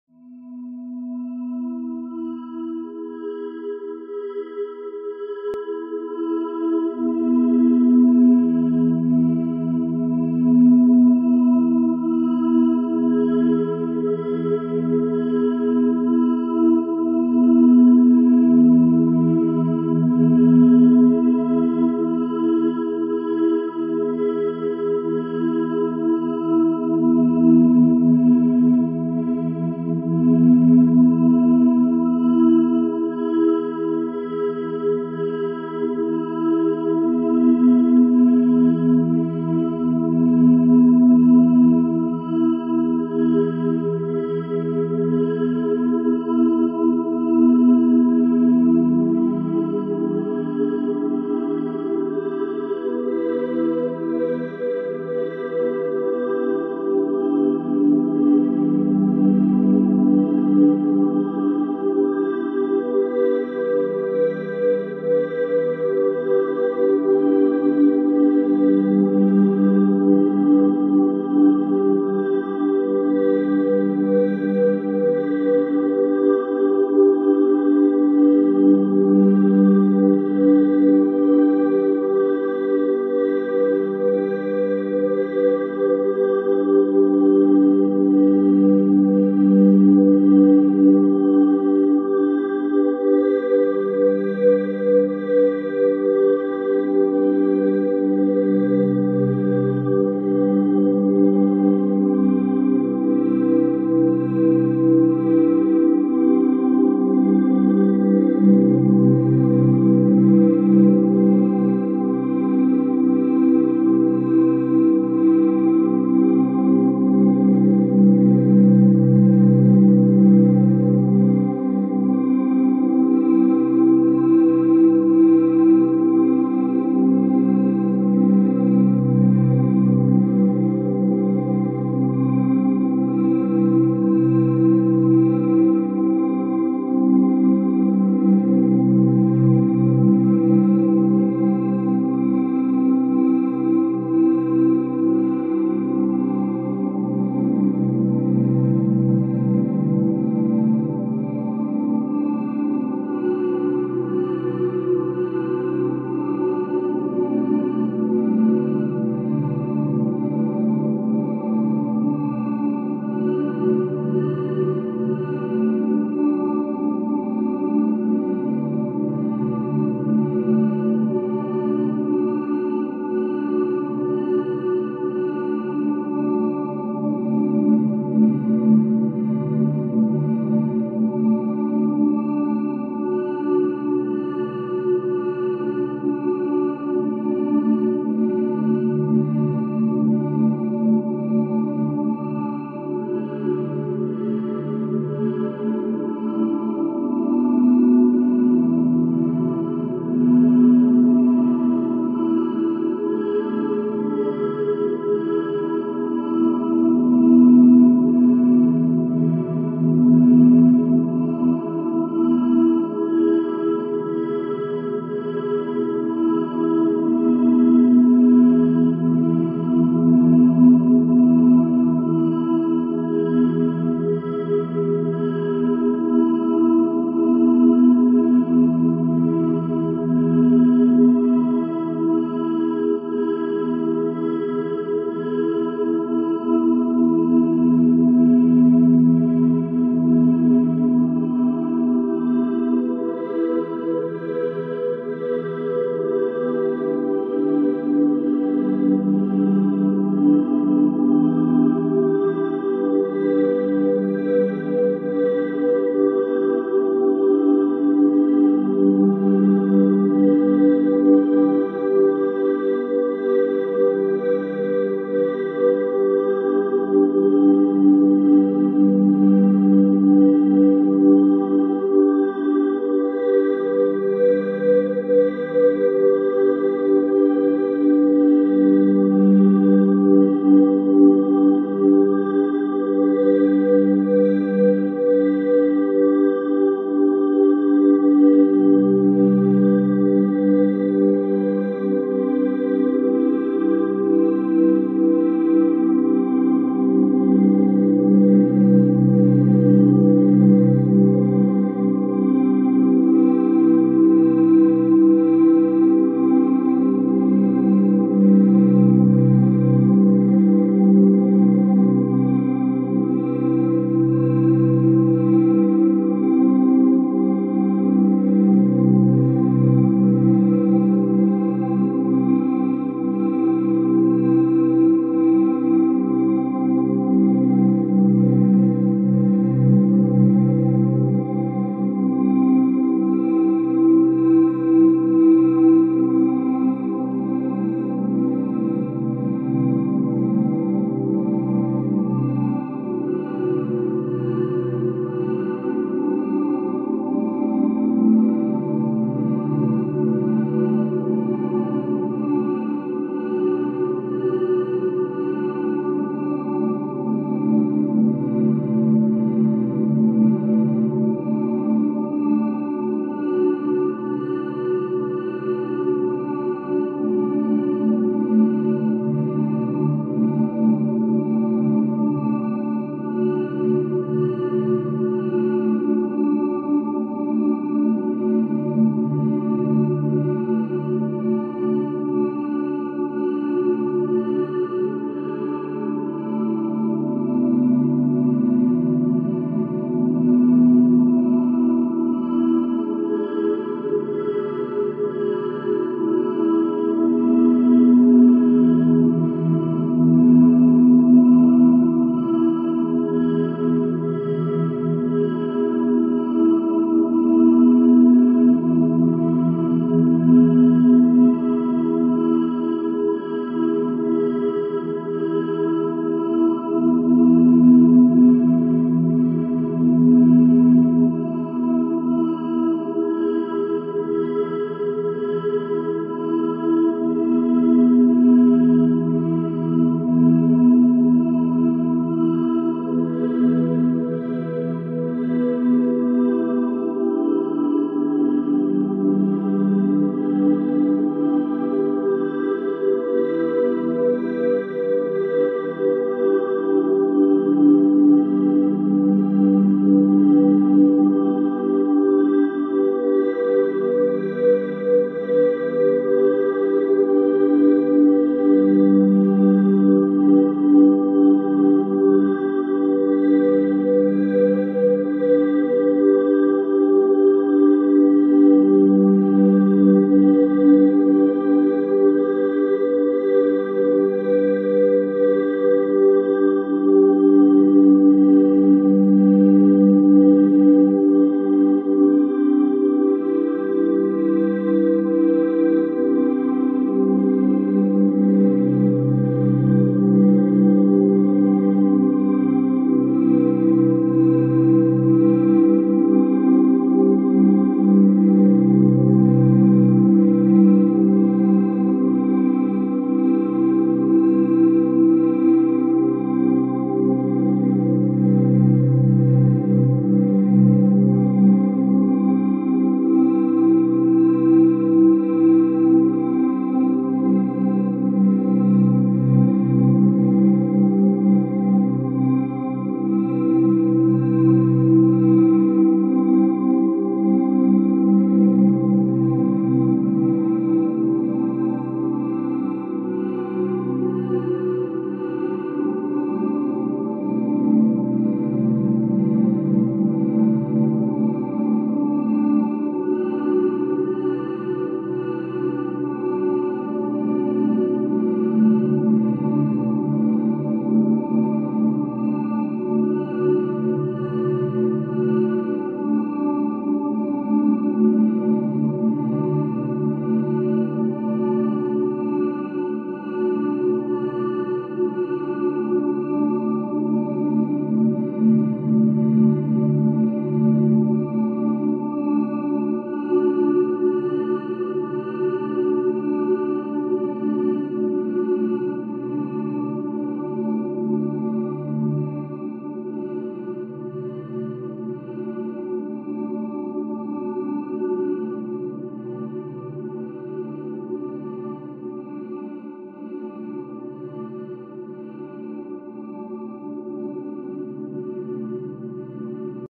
Subliminal - Cleanse Birth Chart + Remove ALL negative energies
This is a subliminal to remove all negative energies from your birth chart and manifest a prosperous life.
this sub was requested by Rashmi N.